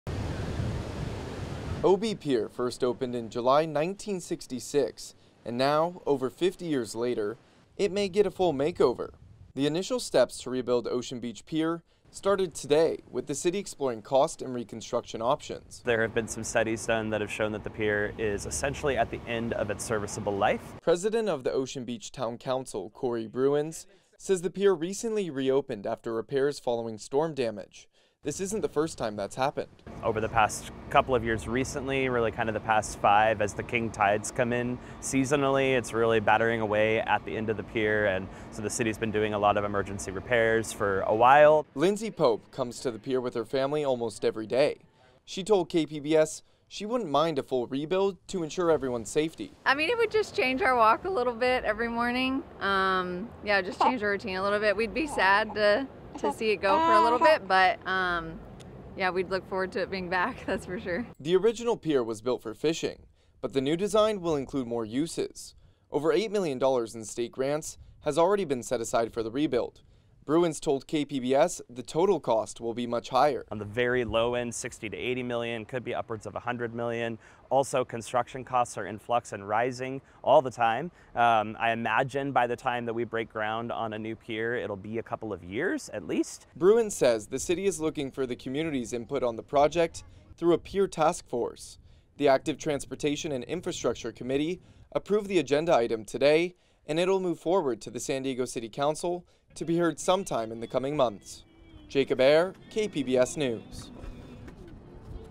City Council accepts state money for study to replace Ocean Beach Pier
Ocean Beach pier is an iconic part of San Diego’s coast line, but it may soon be torn down and fully rebuilt. KPBS Reporter Jacob Aere says the project will cost tens of millions of dollars.